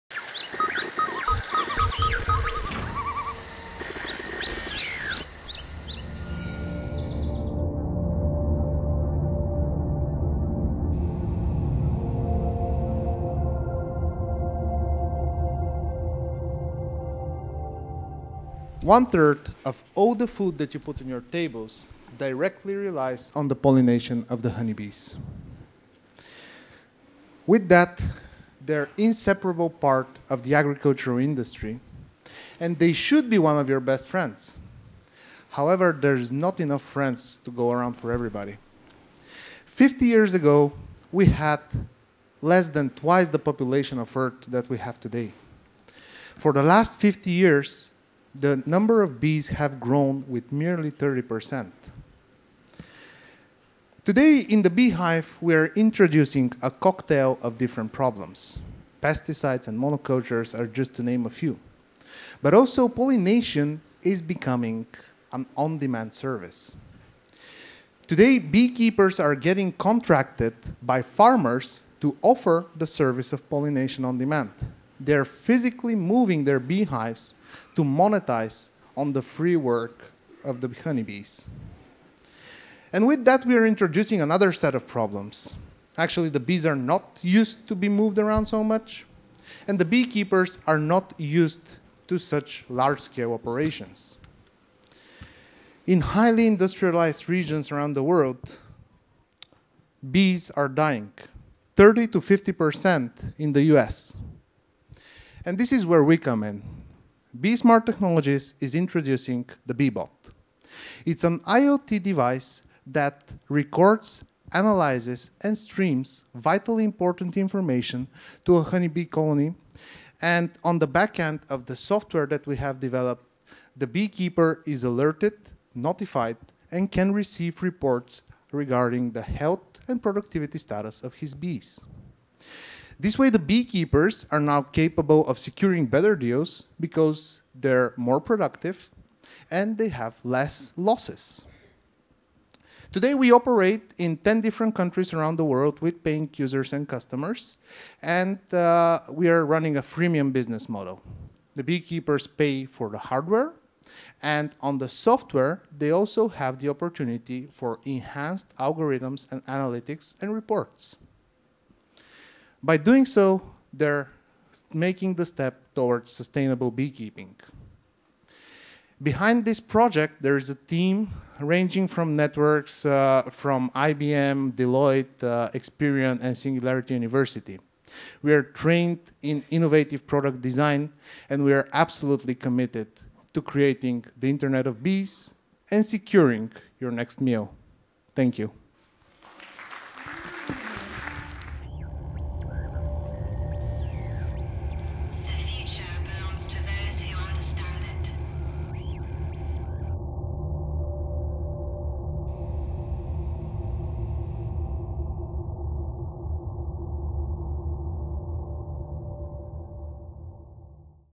Bee Smart at the Hello Tomorrow Summit 2017
Bee Smart Technologies was a finalist in the Food and Agriculture track of the Global Challenge during the Hello Tomorrow Summit 2017. Our founders and CEO Sergey presents the company and our signature product the Beebot during his pitch.